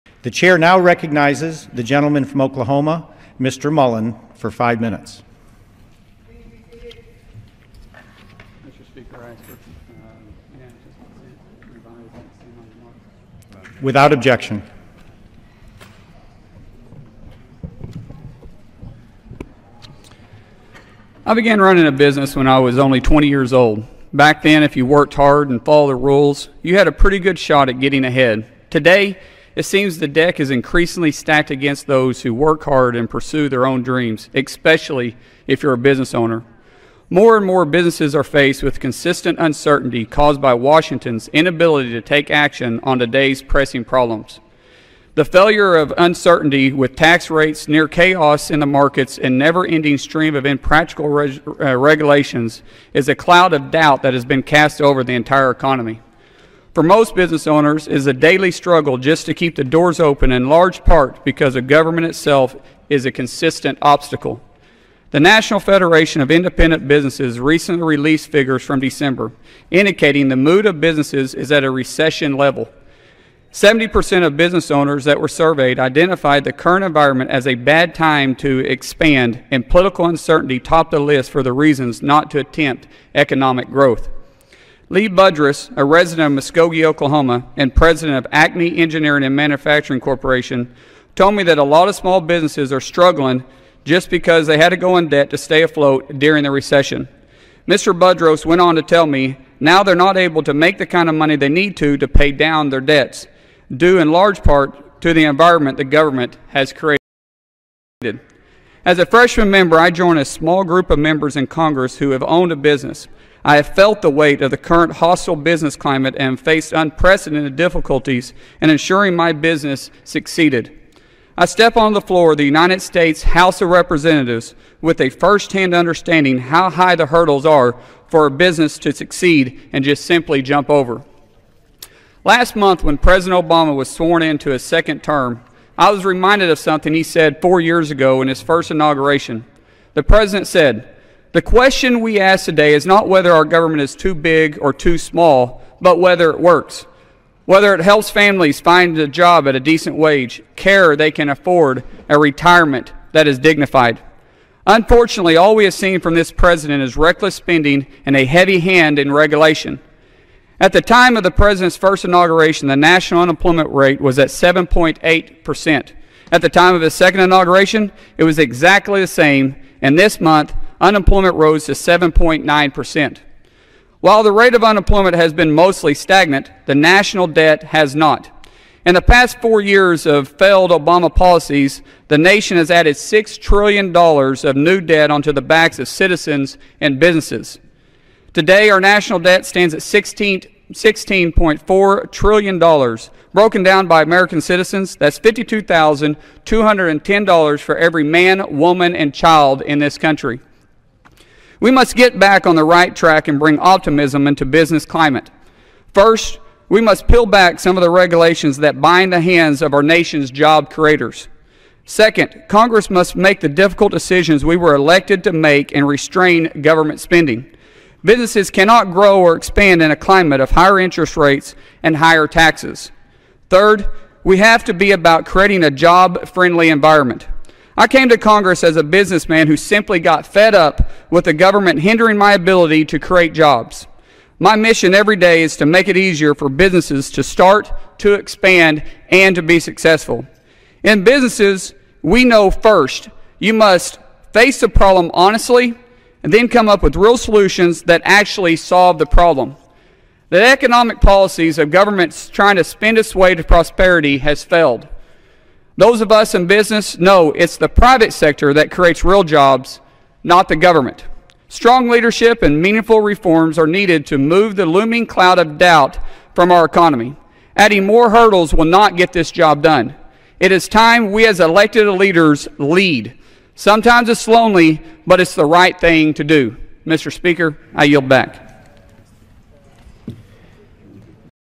Congressman Mullin floor speech - Washington Doesn't Create Jobs
I began running a business when I was only 20 years old. Back then if you worked hard and followed the rules; you had a pretty good shot at getting ahead.

Today it seems the deck is increasingly stacked against those who work hard and pursue their own dreams, especially if you are a business owner.

More and more, businesses are faced with constant uncertainty caused by Washington's inability to take action on today's pressing problems. This failure results in uncertainty with tax rates, near chaos in the markets and a never ending stream of impractical regulations. A cloud of doubt has been cast over our entire economy. For most business owners, it is a daily struggle just to keep the doors open in large part because government itself is a constant obstacle.

The National Federation of Independent Businesses recently released figures from December's "Small Business Optimism Index" indicating the mood of businesses is at a recession level.  Seventy percent of the business owners that were surveyed identified the current environment as a bad time to expand and political uncertainty topped the list for reasons not to attempt economic growth.

Lee Buddrus, a resident of Muskogee, Oklahoma and President of Acme Engineering & Manufacturing Corporation told me that a lot of small businesses are struggling because they had to go into debt just to stay afloat during the recession.   Mr. Buddrus went on to tell me, "Now they are not able to make the kind of money they need to pay down their debts" due in large part to the environment the government has created.

As a freshman member, I join a small group of members in Congress who have owned a business. I have felt the weight of the current hostile business climate and faced unprecedented difficulty in ensuring my business succeeded. I step onto the floor of the U.S. House of Representatives with a first-hand understanding how high the hurdles are that a business must jump over in order to succeed.

Last month when President Obama was sworn in to his second term, I was reminded of something he said just four years ago, in his first inauguration.  The President said:

"The question we ask today is not whether our government is too big or too small, but whether it works — whether it helps families find jobs at a decent wage, care they can afford, a retirement that is dignified."

Unfortunately, all we have seen from this President is reckless spending and heavy handed regulation.

At the time of the President's first inauguration the national unemployment rate was 7.8 percent.  At the time of his second inauguration, it was exactly the same and this month, unemployment rose to 7.9 percent. While the rate of employment has been mostly stagnating, the national debt has not. In the past four years of failed Obama policies the nation added $6 trillion of new debt onto the backs of citizens and businesses. Today, our national debt stands at $16.4 trillion. Broken down by American citizen that's $52,210 for every man, woman and child in this country.

We must get back on the right track and bring optimism into the business climate.  First, we must peel back some of the regulations that bind the hands of our nation's job creators.  Second, Congress must make the difficult decisions we were elected to make and restrain government spending. Businesses cannot grow or expand in a climate of higher interest rates and higher taxes. Third, we have to be about creating a jobs friendly environment. I came to Congress as a businessman who got fed up with government hindering my ability to create jobs. My mission every day is to make it easier for businesses to start, to expand and to be successful.

In business, we know that first, you must face the problem honestly and then come up with real solutions that actually solve those problems.

The economic policy of having government spend our way to prosperity has failed. Those of us in business know it is the private sector that creates real jobs, not the government.

Strong leadership and meaningful reforms are needed to remove the looming cloud of doubt from our economy. Adding more hurdles will not get this job done.

It is time that we as elected leaders roll up our sleeves and get to work.